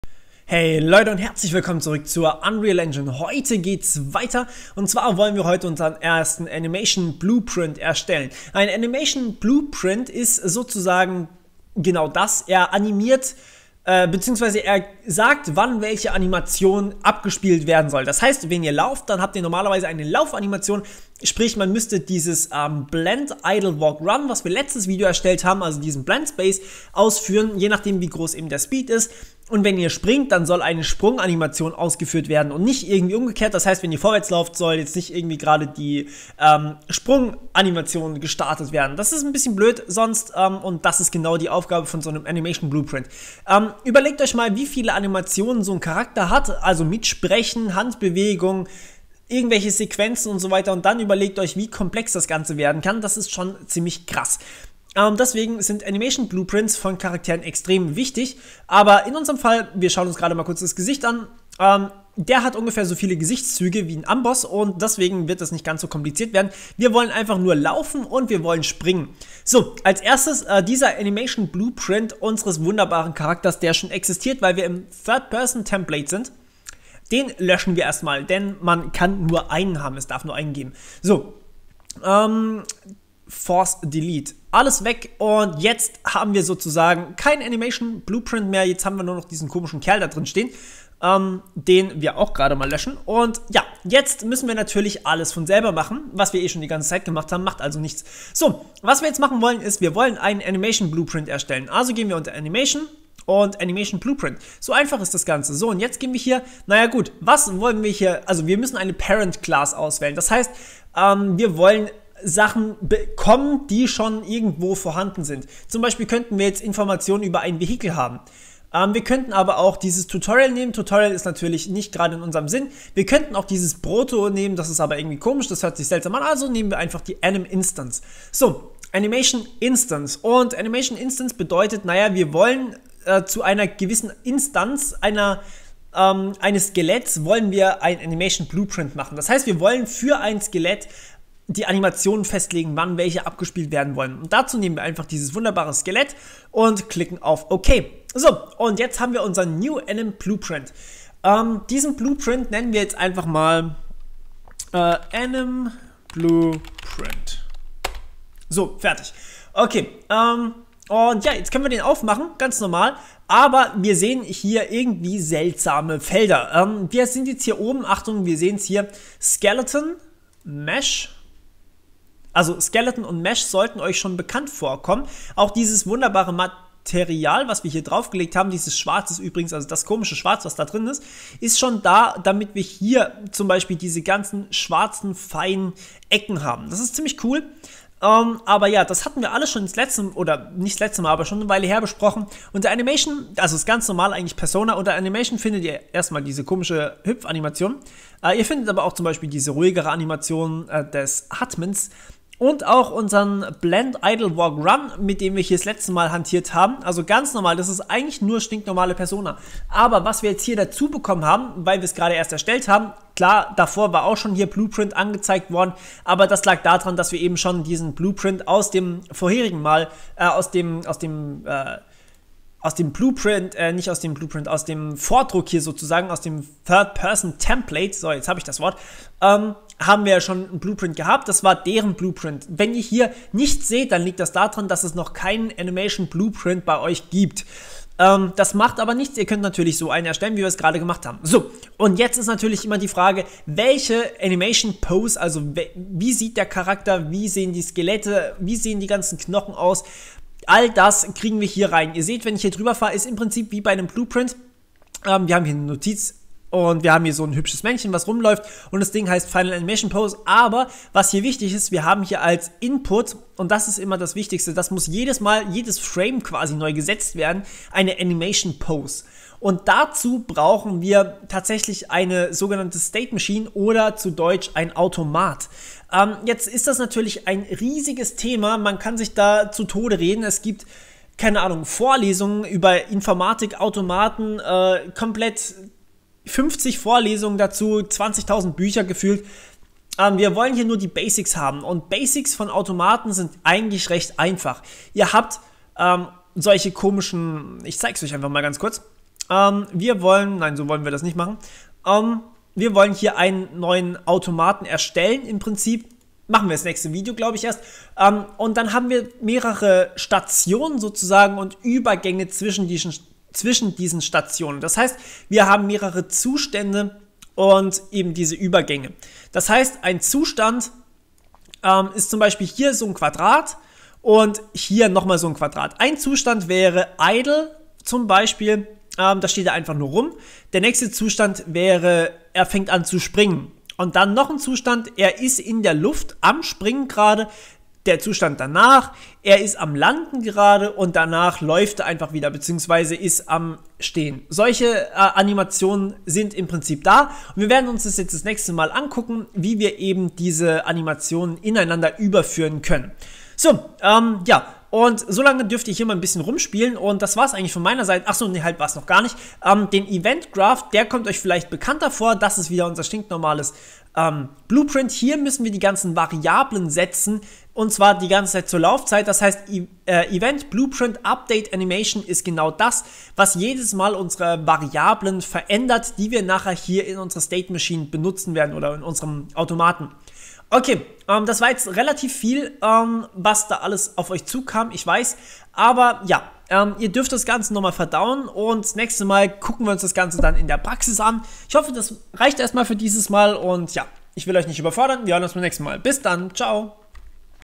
Unreal Engine 4 Tutorial #27 - Animation Blueprints [DEUTSCH/GERMAN]
In diesem Tutorial fangen wir mit dem Animation Blueprint an, der unseren Character animiert.